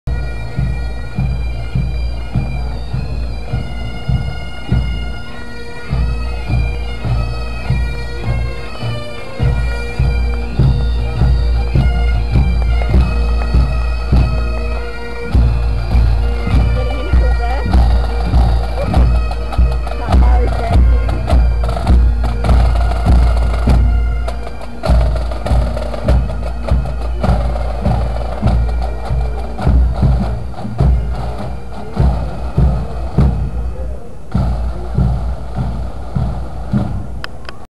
ANZAC Day 2011 - National Servicemens Association Pipes and Drums
In this video, we see a pipe band marching past the crowds during the Queensland ANZAC Day March in Brisbane City. They are the National Servicemens Memorial Pipes and Drums and they consist of current and former members of the Australian Defence Force as well as members of the National Servicemens Association and Queensland Emergency Services e.g. QPS, QFES and QAS.

Filmed on Monday April 25, 2011 (ANZAC Day)